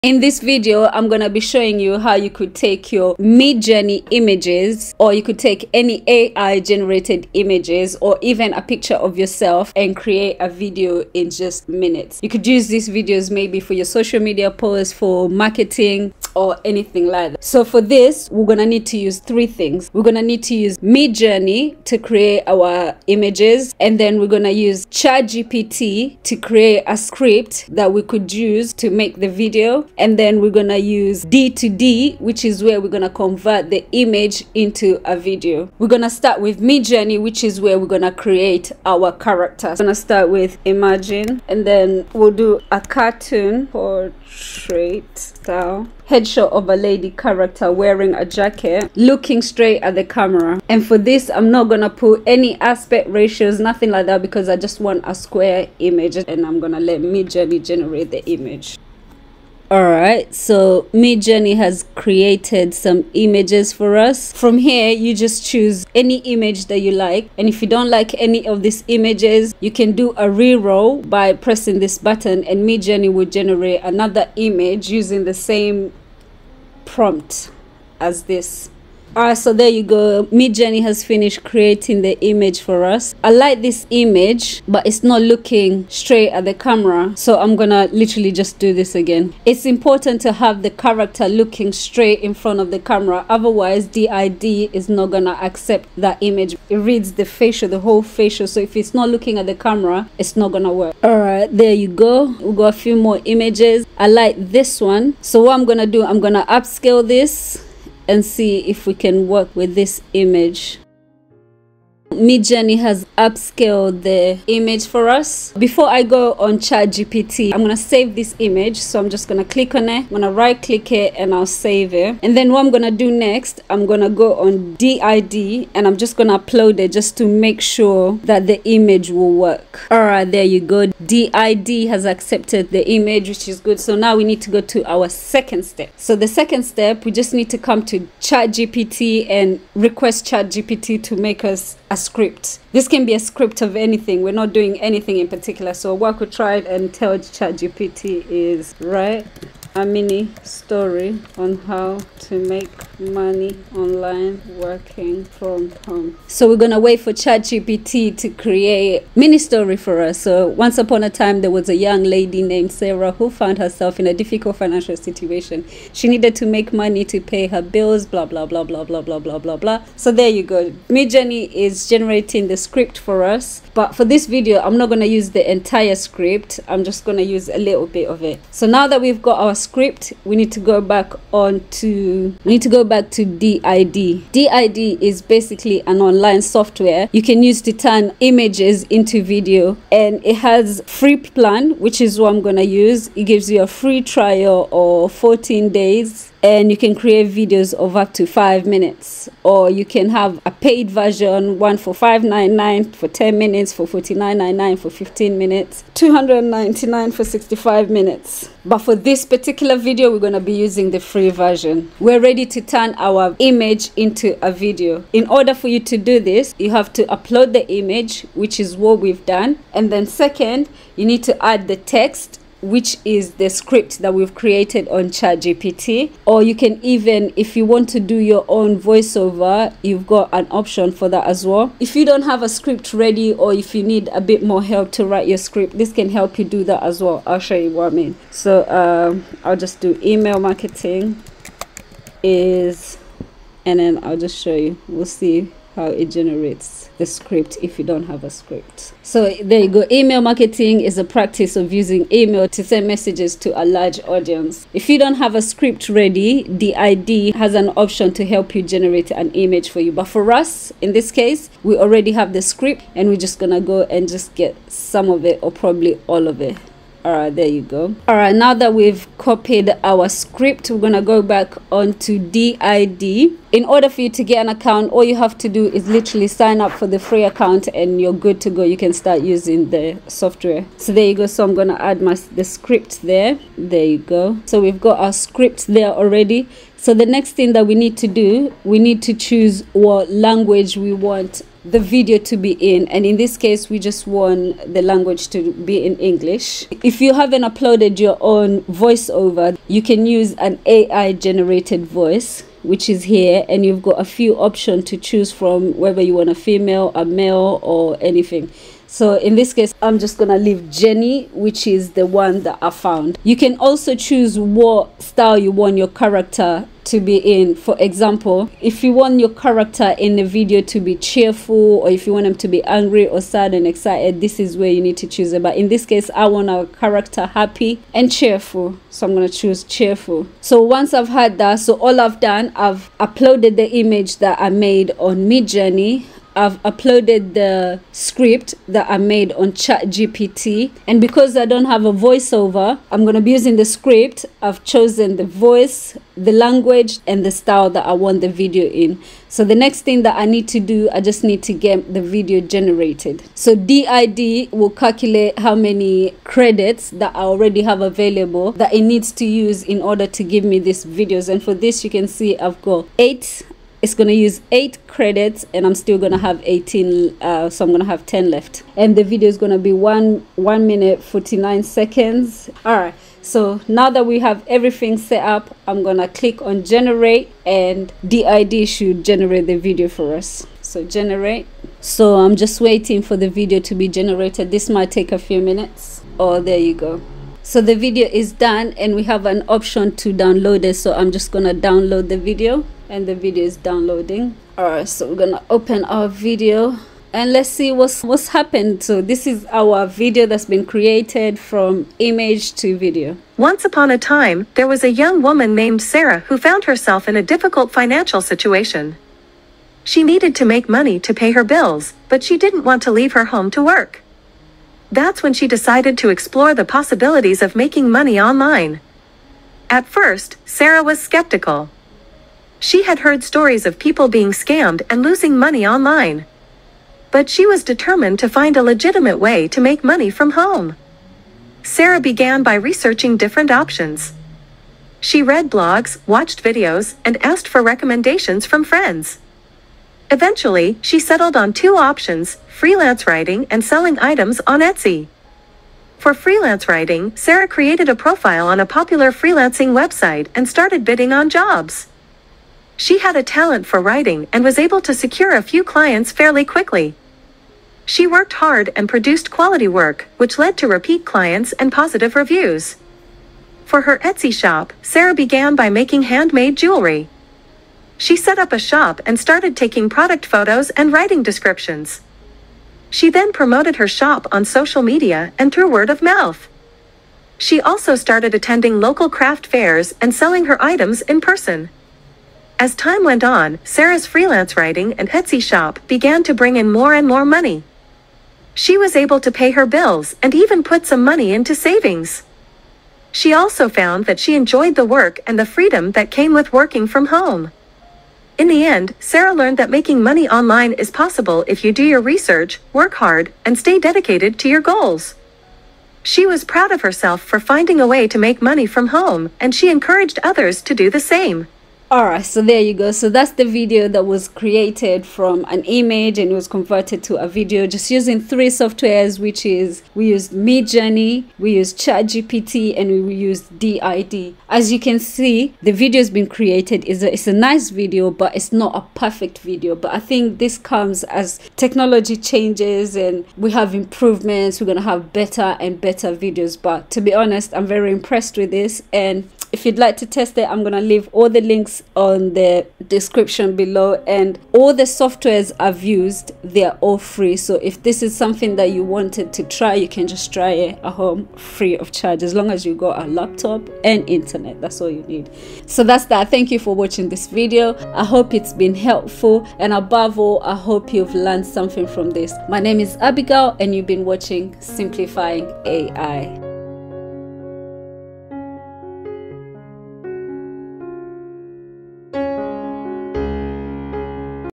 How To Animate A MidJourney Image To Video | Ai Image To Video
In this video, we'll explore how to take AI-generated images from Midjourney and turn them into animated videos using free software.

Midjourney is a fantastic AI platform that allows you to create stunning images without needing any design skills. However, what if you want to take those images to the next level and turn them into eye-catching animated videos? That's where this tutorial comes in.

We'll start by showing you how to export your Midjourney images and import them into a free video editing software, such as D-ID, aswel as using ChatGPT for a script. Then, we'll walk you through the steps of animating your images using midjourney, ChatGPT and D-ID

You don't need any prior experience in video editing to follow along with this tutorial. We'll explain everything in a clear and concise manner, and you can pause and rewind the video as many times as you need.

By the end of this tutorial, you'll have the skills and knowledge to turn your Midjourney images into professional-looking animated videos, all using free software. So, grab your AI-generated images, and let's get started!